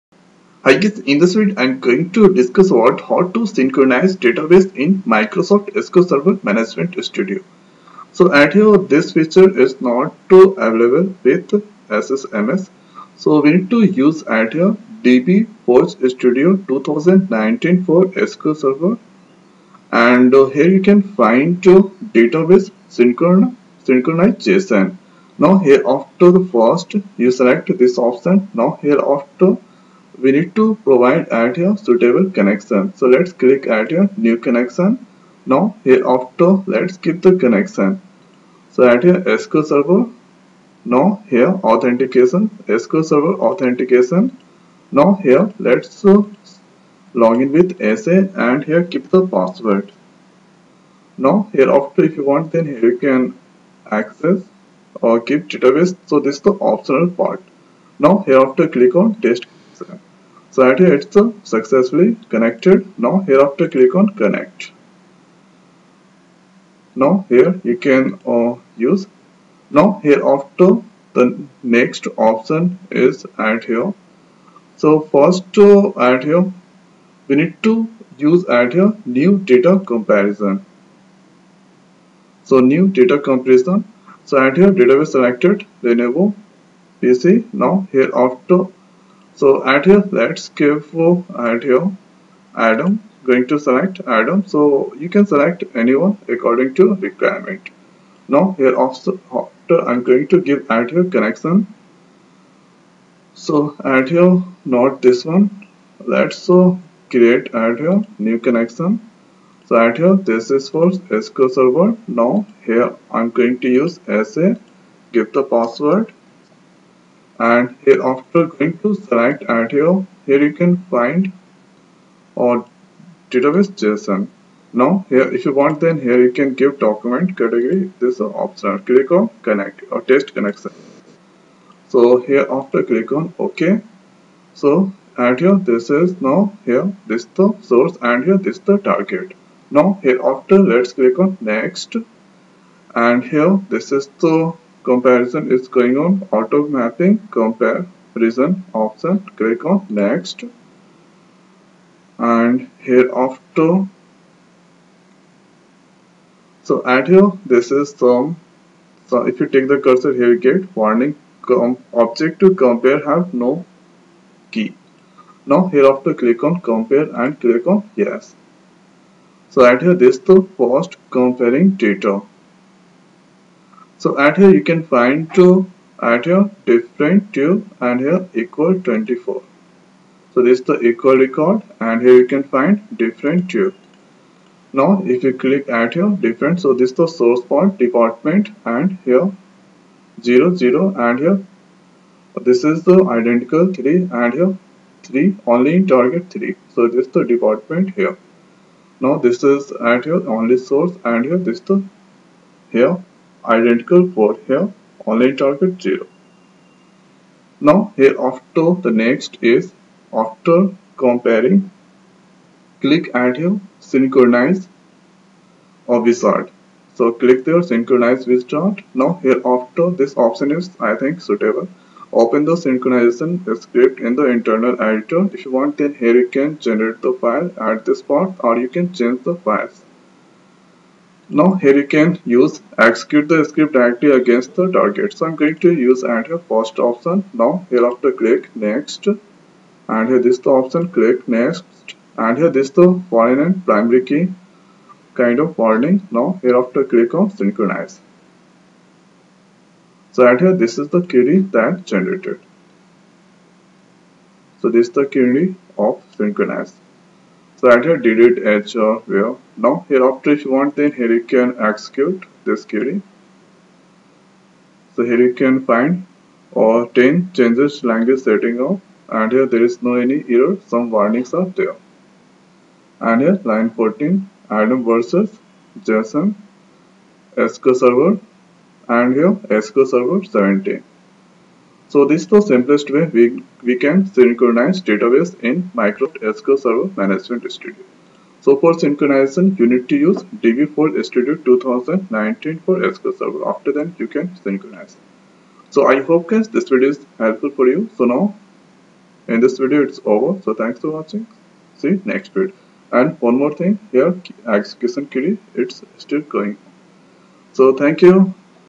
Synchronize Database in SQL Server
comparing and synchronizing two database in sql server